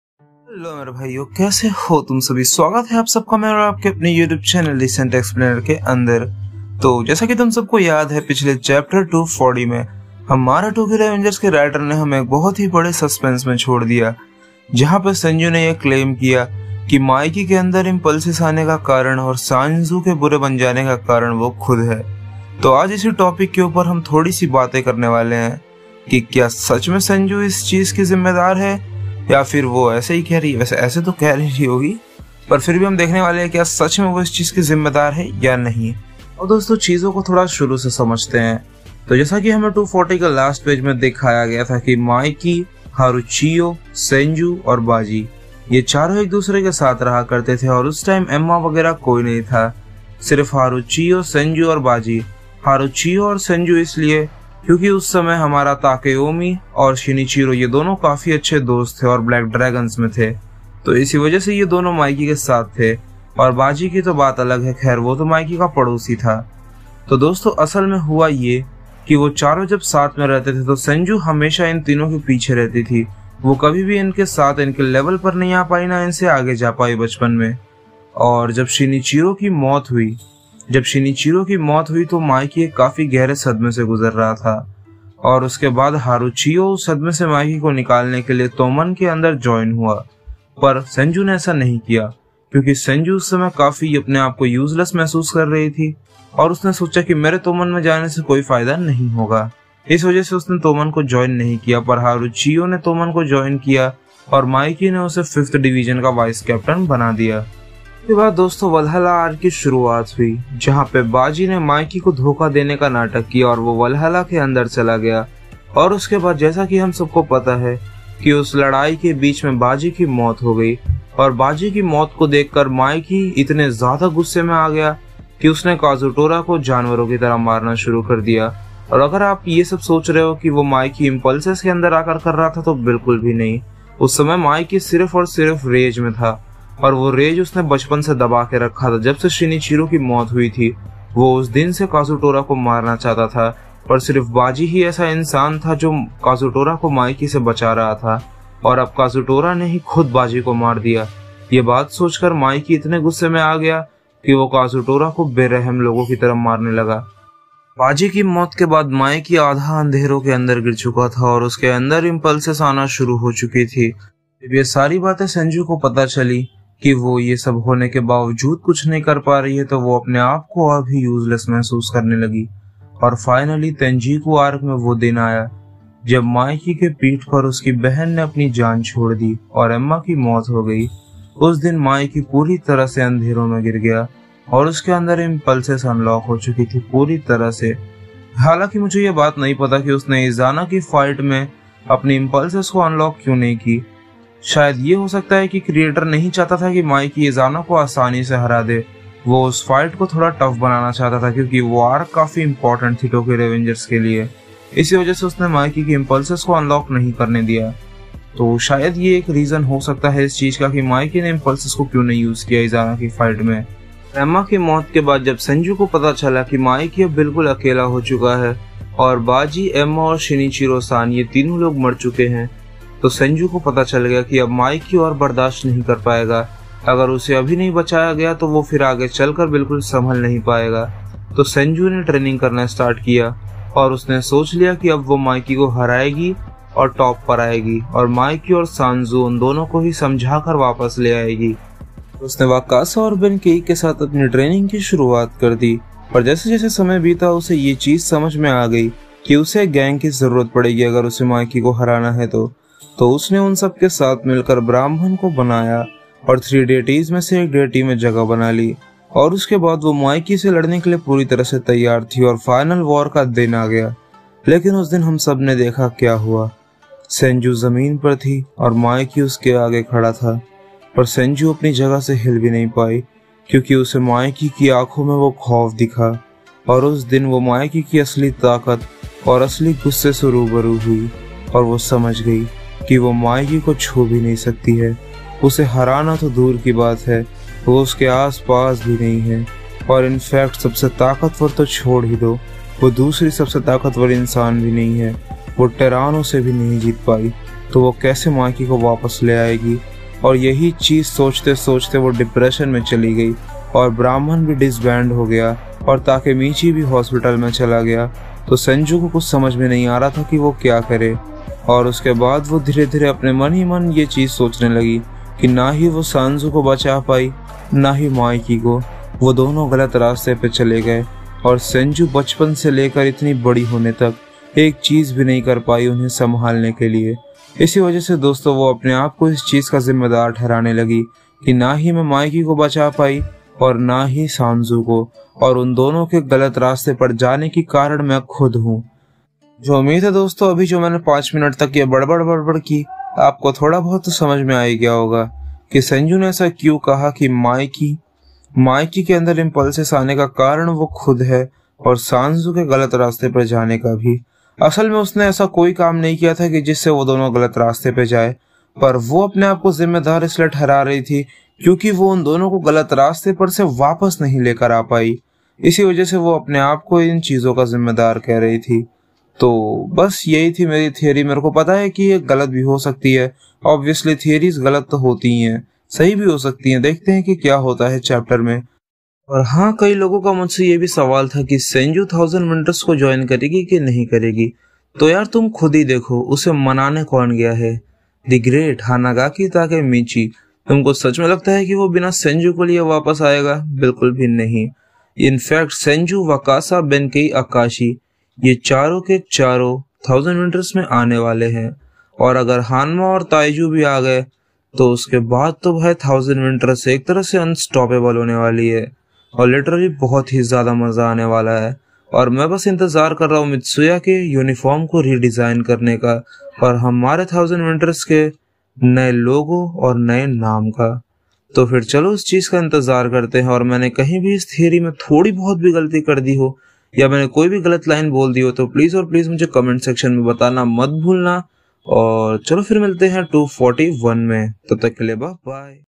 Tokyo Revengers is senju really the reason for mikey's impulses by (descent explainer)
TOKYO revengers